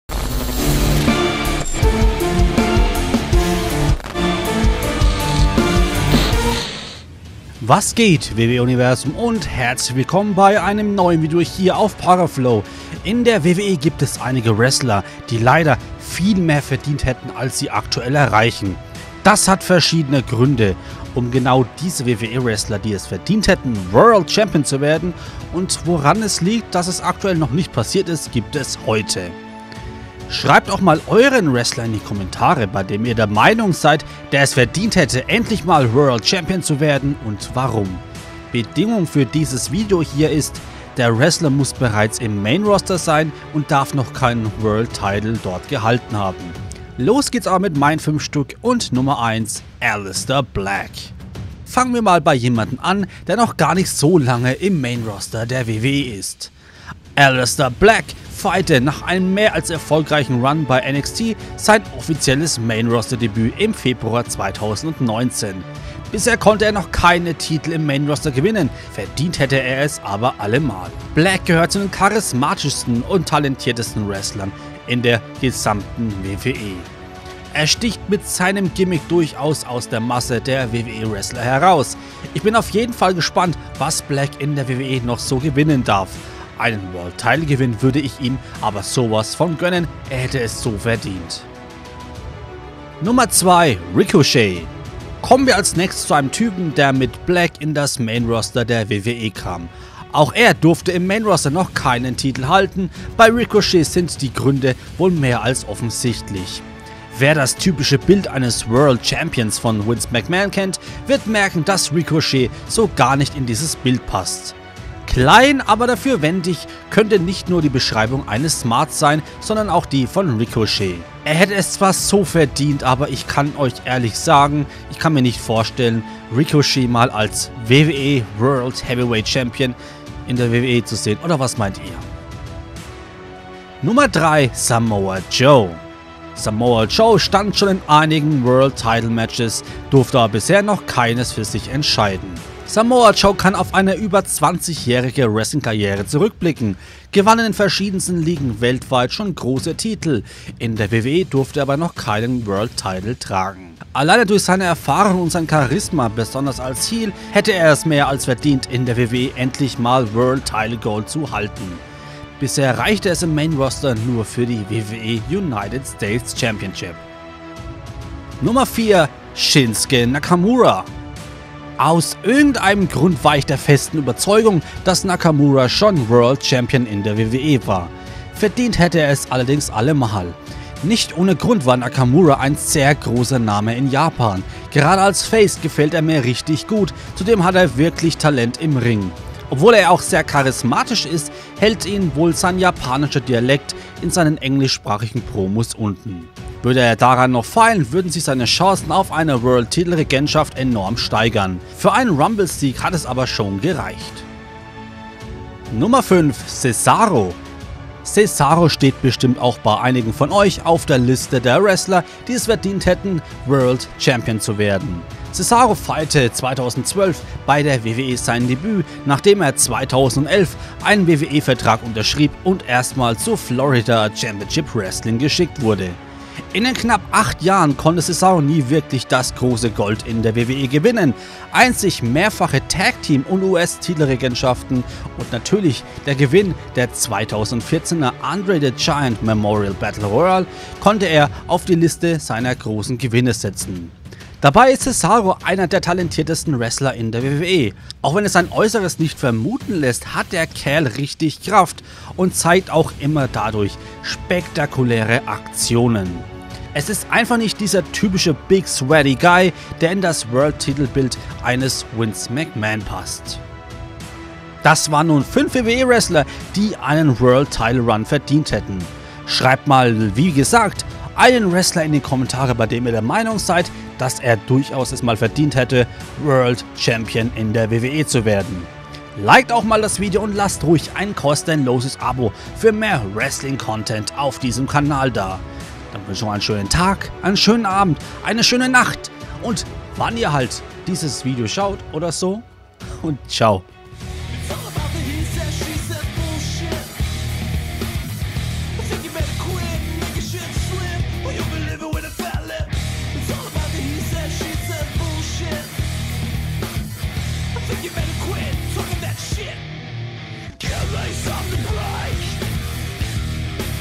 5 WWE-Wrestler die WORLD CHAMPION sein sollten (Deutsch/German) || Paraflow
Es gibt einige WWE Wrestler in der WWE, die es verdient hätten mal World Champion (WWE Universal Champion & WWE World Heavyweight Champion) zu werden. Davon gibt es heute 5 Stück.

Equipment
✗2ter Bildschrim
✗3ter Bildschrim